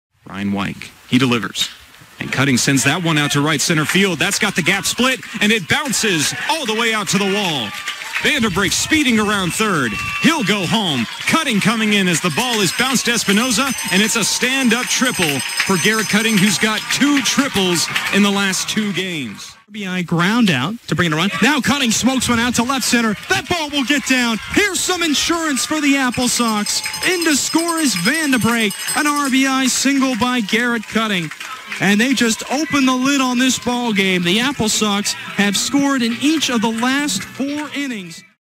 Garrett Cutting's 2nd Straight 2-Hit Night 8.5.21
Garret Cutting picks up his second consecutive two-hit night as he triples in one run and then singles in another against the Port Angeles Lefties on Aug. 5, 2021, at Paul Thomas Sr. Stadium.